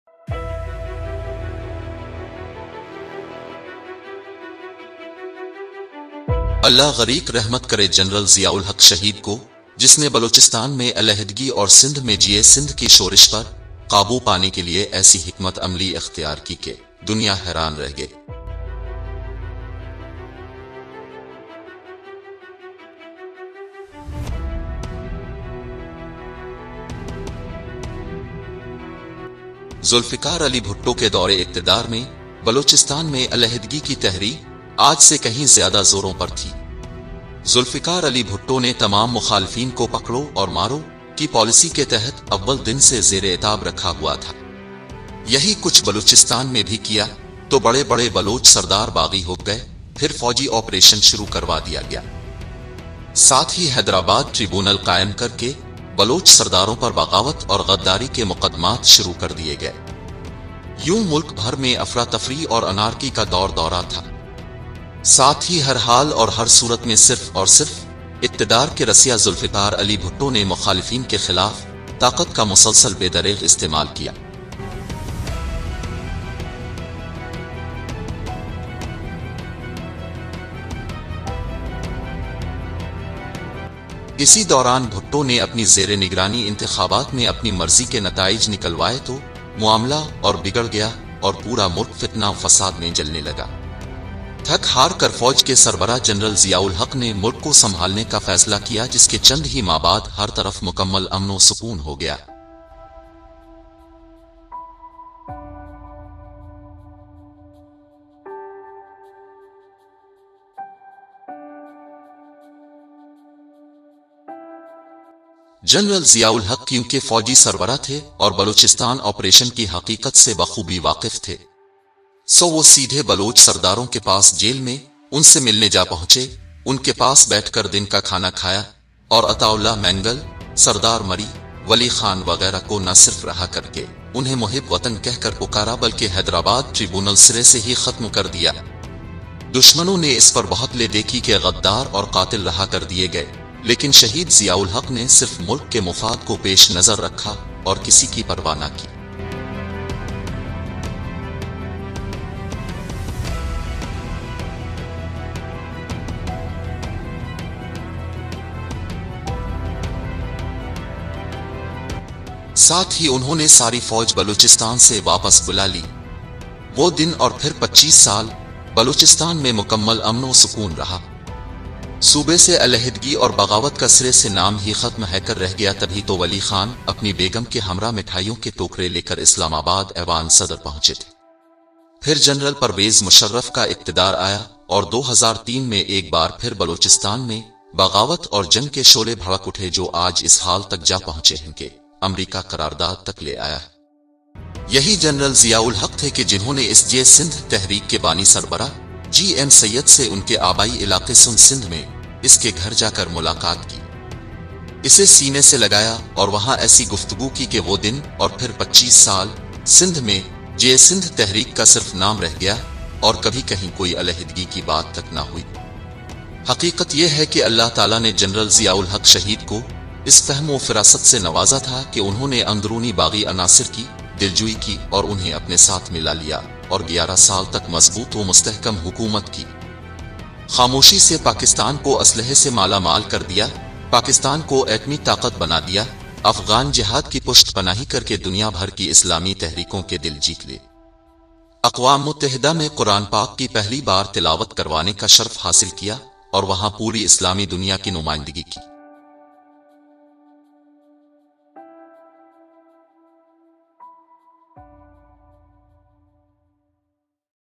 General Zia Ul Haq Shaheed Hero Number One / Ijaz Ul Haq
The video explores General Zia-ul-Haq Shaheed's political maneuvers during his presidency in Pakistan from 1978 to 1988. Specifically, it focuses on his release of political leaders who were declared rebels during the previous administration of Zulfikar Ali Bhutto. The video also delves into General Zia-ul-Haq's efforts to embrace disgruntled Baloch leaders, including GM Syed, to quell Baloch separatism.

Overall, the video offers a nuanced exploration of General Zia-ul-Haq's political strategies, presenting multiple perspectives and encouraging viewers to critically think about this complex historical figure.